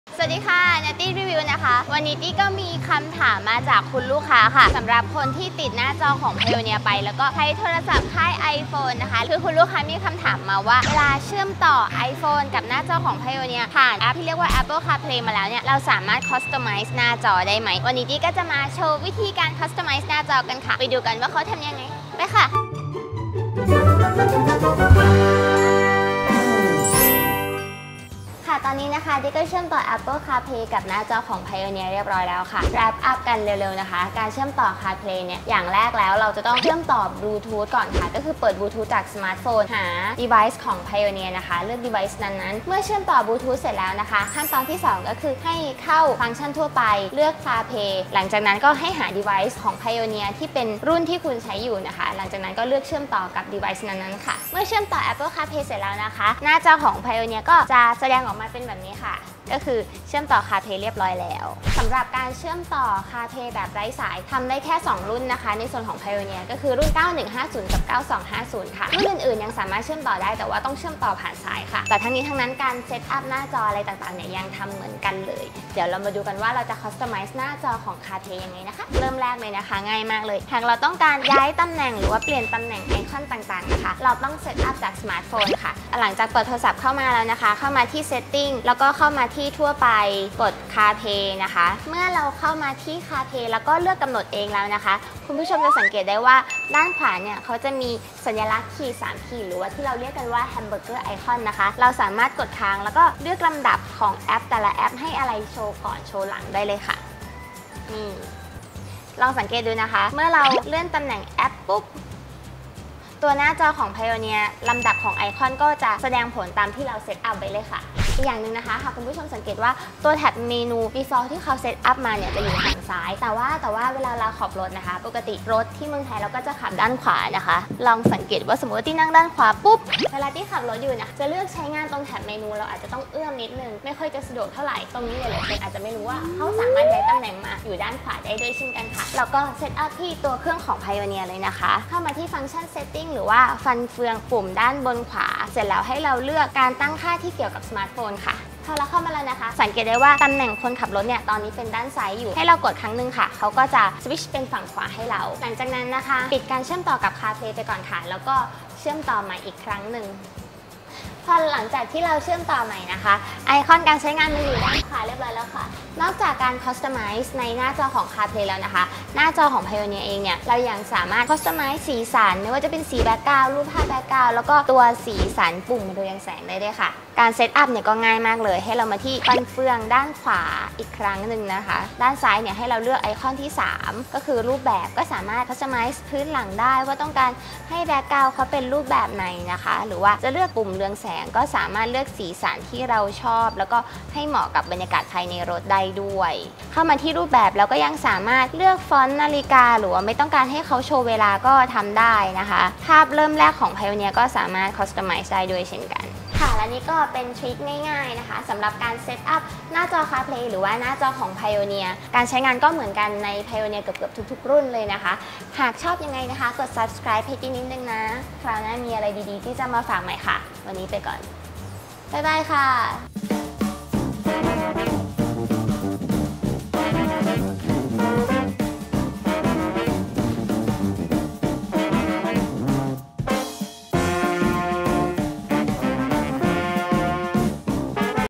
Natty Review วิธี Customize หน้าจอ Application Apple Carplay
⭐️😀 Natty Review 😀⭐️

วิธี Customize หน้าจอ Application Apple Carplay ปรับเปลี่ยน ย้ายตำแหน่ง icon สลับฝั่ง Menu Default ให้เหมาะสมกับการใช้งาน สามารถทำได้ง่ายๆ ใครๆก็ทำได้ !!

ติดตามข้อมูลข่าวสาร เครื่องเสียงรถยนต์ได้ที่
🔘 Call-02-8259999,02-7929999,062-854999